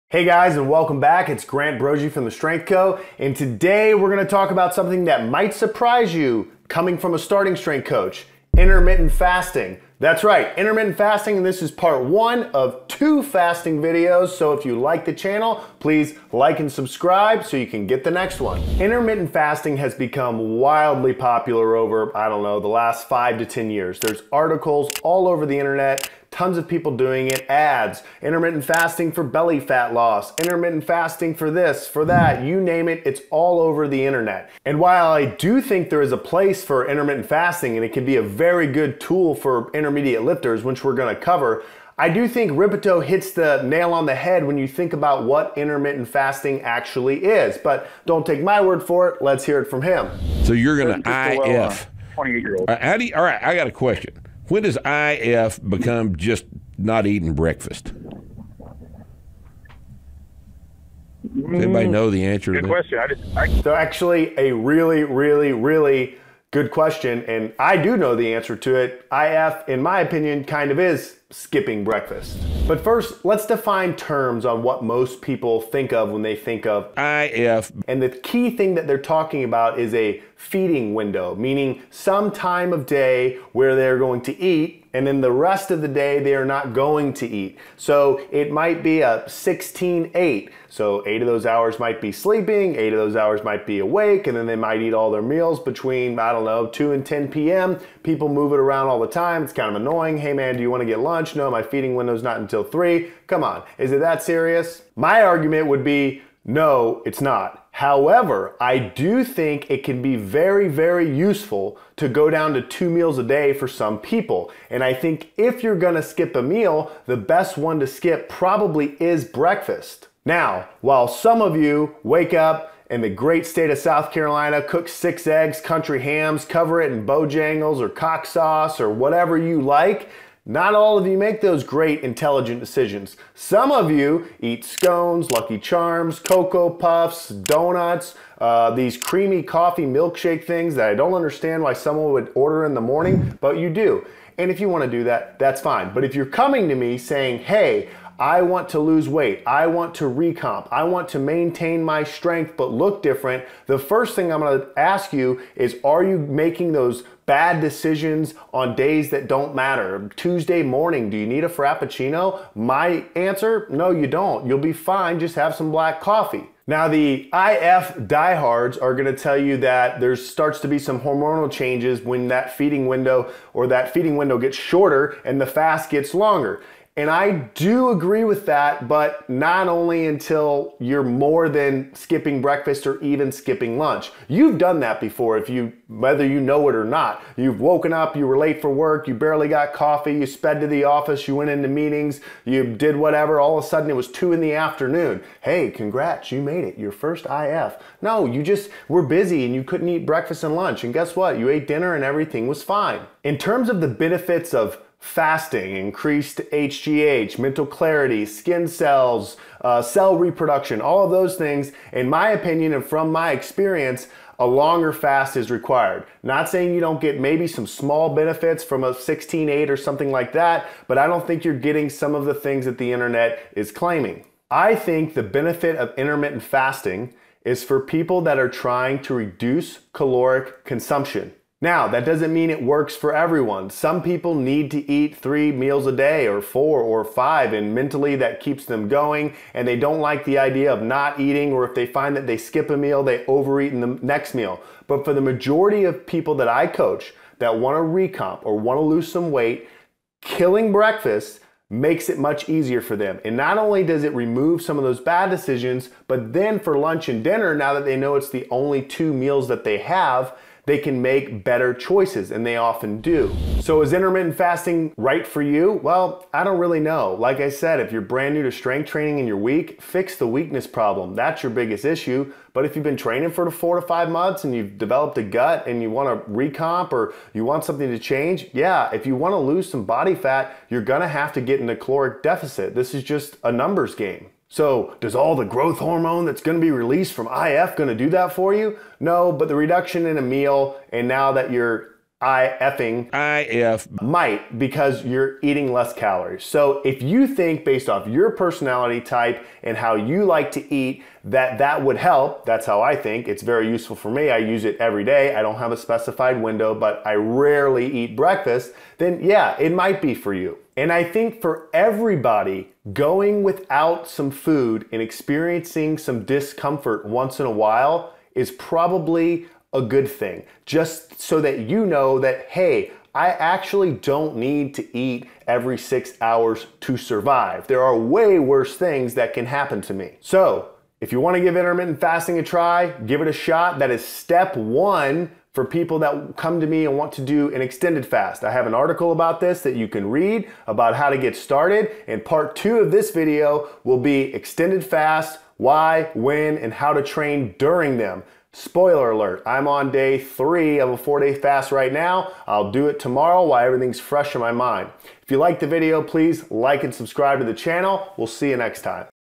The TRUTH About Intermittent Fasting | Starting Strength Coach Explains
Is IF fake? Is it just skipping breakfast? Starting Strength Coach Grant Broggi explains what IF is, how to do it, and how you might want to incorporate it into your diet.